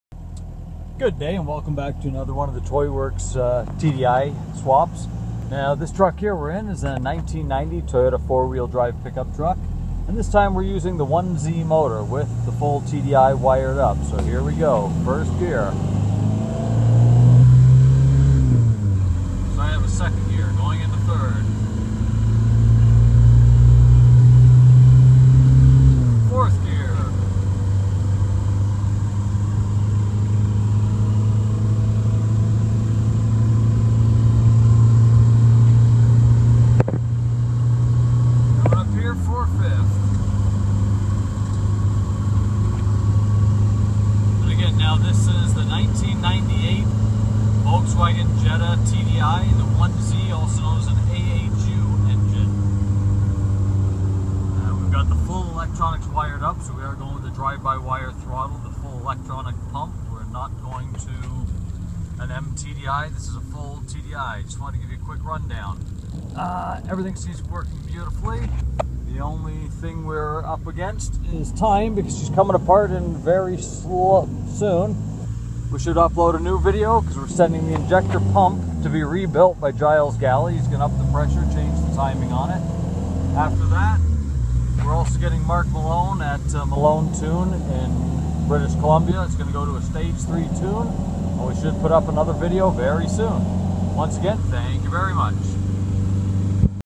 Toyota diesel conversion 1.9 TDi road test 1
1Z / AHU conversion in 93 Toyota 4X4 toad test results before upgrades. Stock 95-99 VW 1.9 1Z/AHU TDi engine